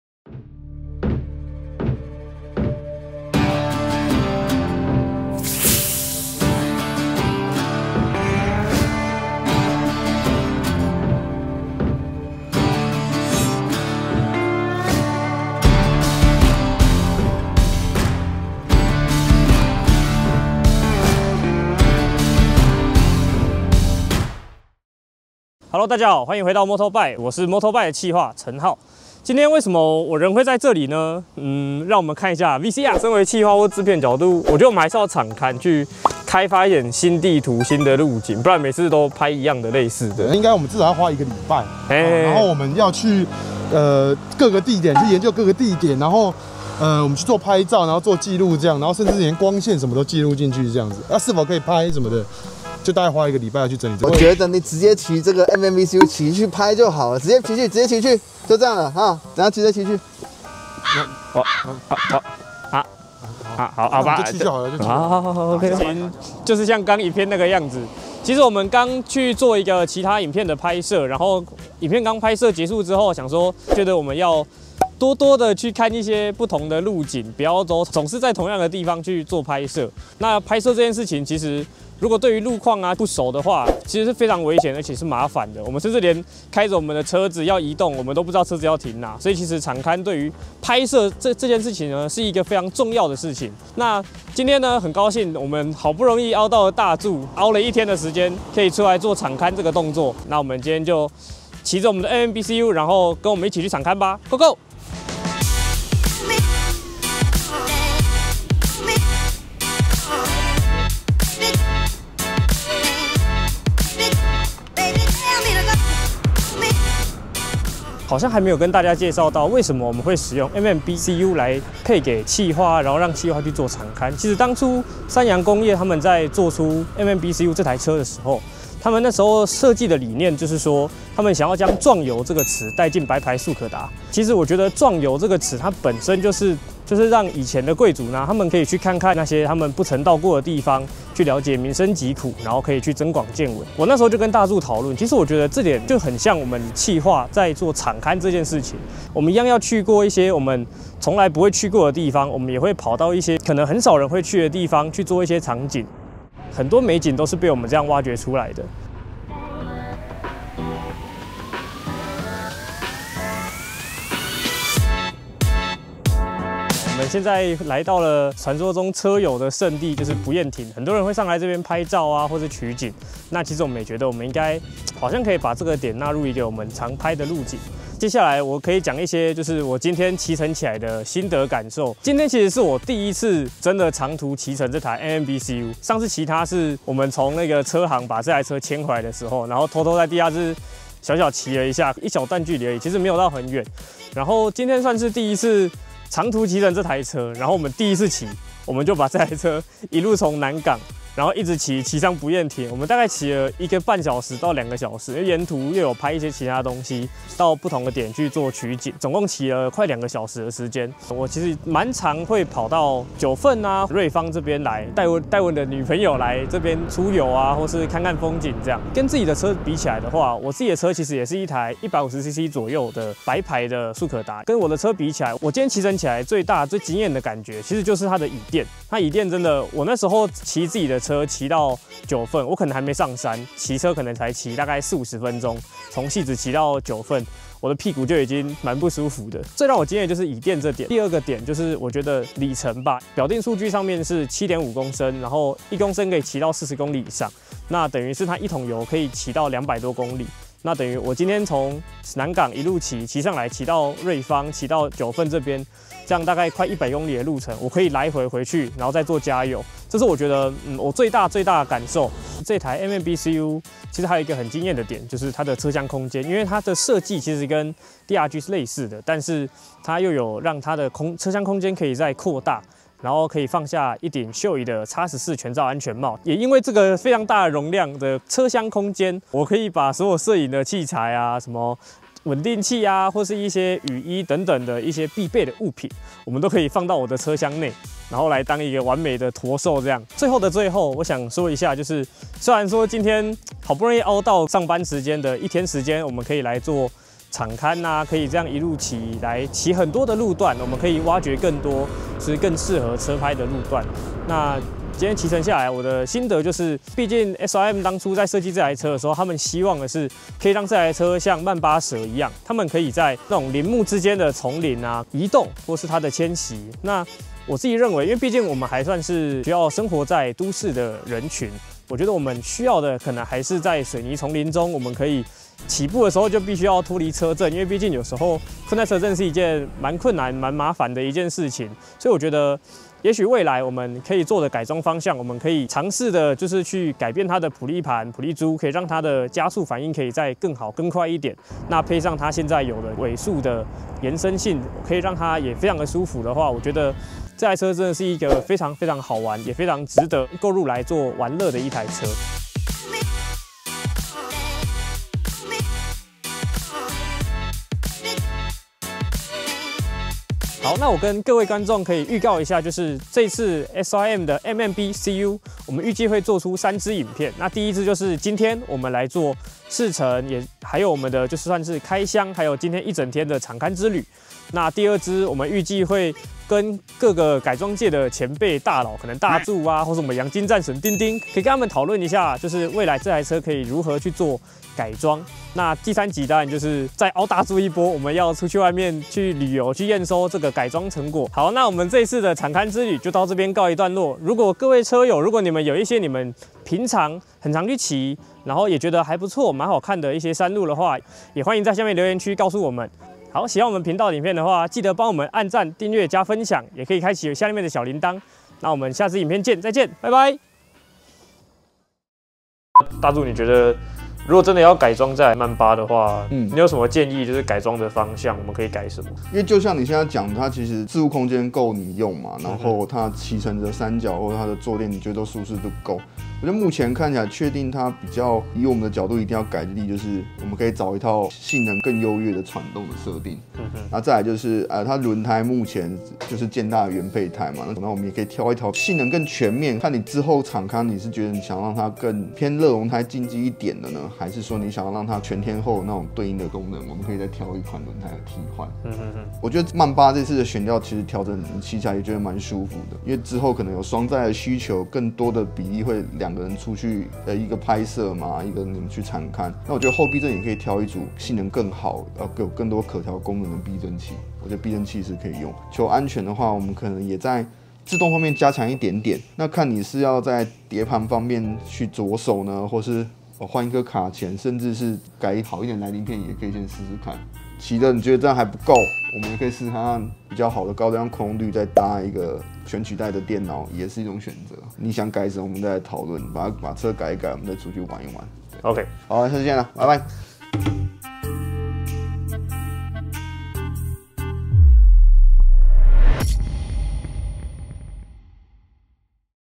凹了一台機車還不夠？再凹一天去場勘❌ (偷懶⭕️ ） | SYM MMBCU場勘記｜【特別企劃】
更多詳細報導

特殊動態畫面器材：Insta 360 ONE RS ＆ ONE X2